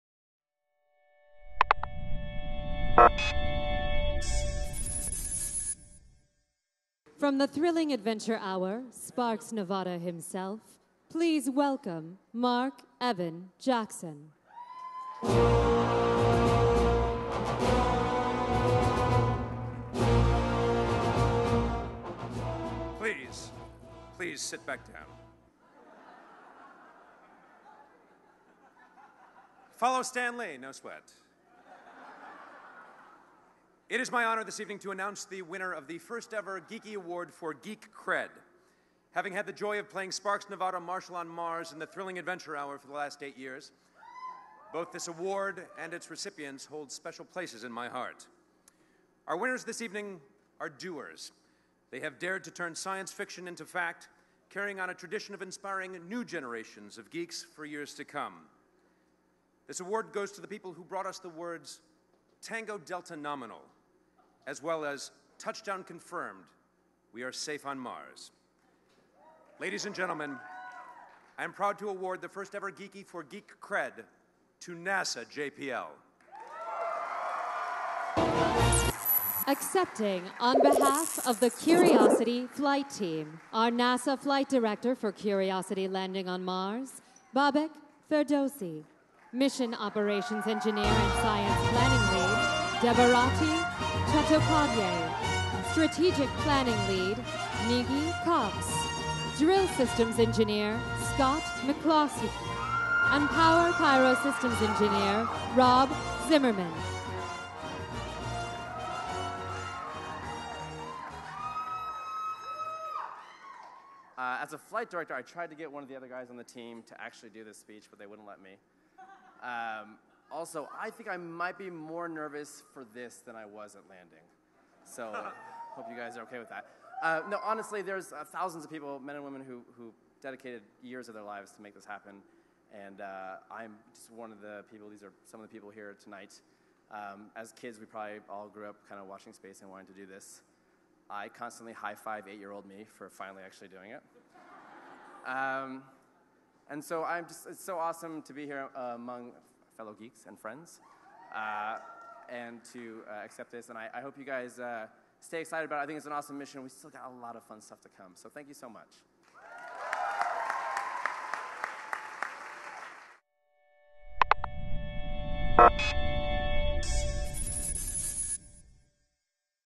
The Geekie Awards 2013: NASA / JPL Mars Curiosity Rover Receives the 'Geek Cred' Award
The 1st Annual Geekie Awards. Marc Evan Jackson (The Thrilling Adventure Hour) Presents NASA / JPL Mars Curiosity Rover with the 'Geek Cred' Award at the Hollywood Avalon on August 18, 2013.

Accepting on behalf of the Curiosity Flight Team are: NASA Flight Director for Curiosity landing on Mars Bobak; Mission Operations Engineer and Science Planning Lead,
Debarati Chattopadhyay; Strategic Planning Lead, Nagin Cox; Drill Systems Engineer, Scott McCloskey; and Power/Pyro Systems Engineer, Rob Zimmerman.